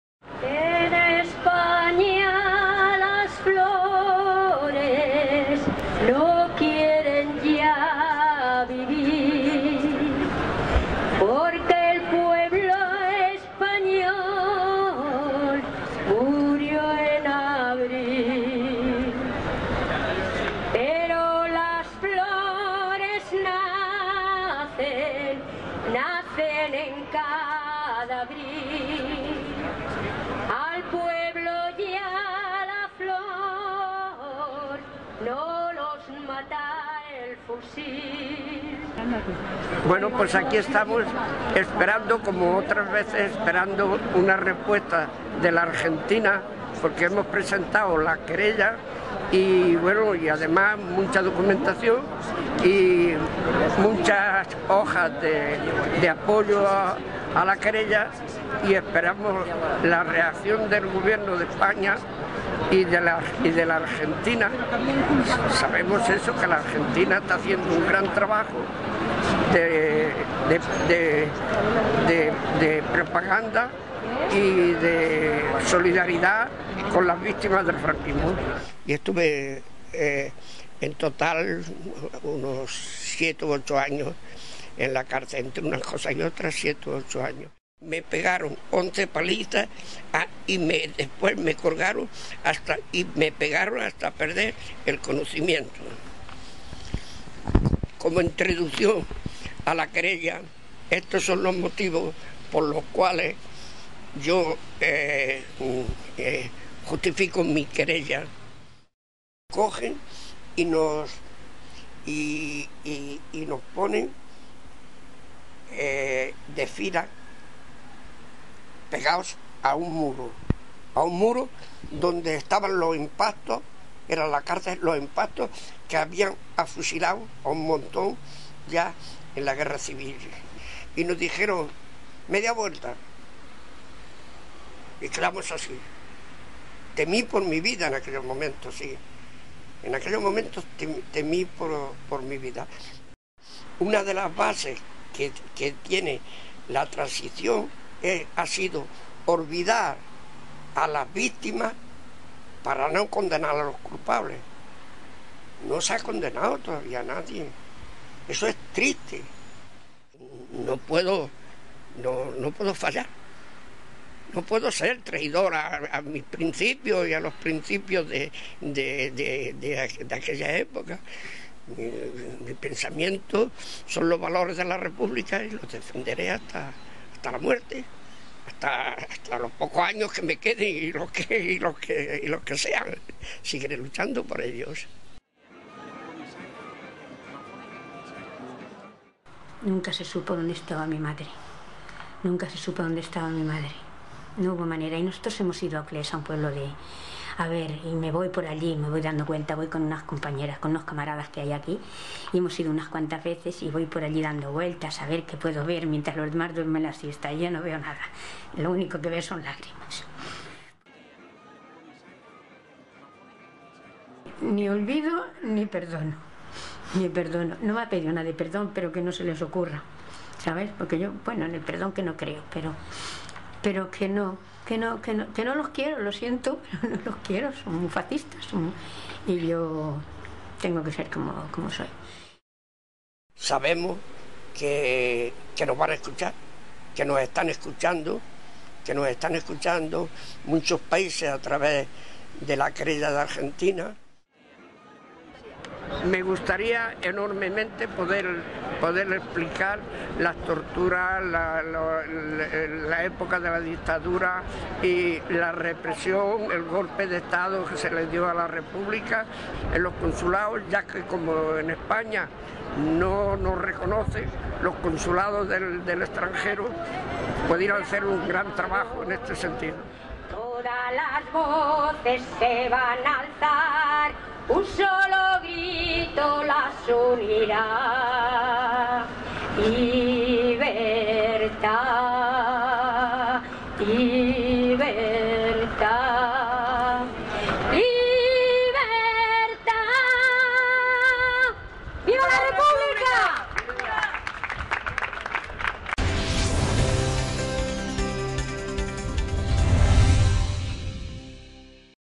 "Sabemos que nos están escuchando gracias a Argentina"
Juan Martínez Martínez y su esposa Paquita, dos veteranos republicanos, se reúnen el último sábado de cada mes junto a otras víctimas del franquismo en la plaza Sant Jaume de Barcelona para reclamar justicia: "Sabemos que ahora nos están escuchando gracias a la querella de Argentina", aseguran esperanzados.

La pareja de ancianos está unida por un intenso amor, fuertes convicciones y una historia que ha dejado heridas aún sangrantes en España: la guerra civil (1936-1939) y la posterior dictadura franquista (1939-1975).

"Me torturaron, estuve en total entre siete y ocho años preso, pero durante lo poco que me quede de vida seguiré defendiendo los valores de La República, por los que luché", dice a Télam Juan, de 86 años.

"A mí me llamaban la niña de la cárcel", apunta Francisca Cruz Puntero, varios años más joven. Paquita siempre lleva con ella dos fotos, la de su madre, que murió en las cárceles franquistas, y la de su tío, que fue fusilado. "Yo no olvido ni perdono", afirma emocionada.

Pasó más de un mes desde que la jueza argentina María Servini de Cubría dictó la primera orden de detención internacional con fines de extradición contra cuatro represores franquista.

Ante la inédita decisión, la justicia española respondió que dos de los reclamados, José Ignacio Giralde Gómez y Celso Galván Abascal habían fallecido. Los otros dos, el ex guardia civil Jesús Muñecas Aguilar, de 74 años, y el ex inspector Jose´Antonio González Pacheco, alías "Billy El Niño", de 67 años, serían citados a testificar.

Las declaraciones tendrán lugar cuando la magistrada argentina confirme su solicitud de extradición aportando toda la documentación necesaria. Servini de Cubría también pidió la apertura de los consultados del país en el exterior para recoger el testimonio de las víctimas y así ampliar su investigación.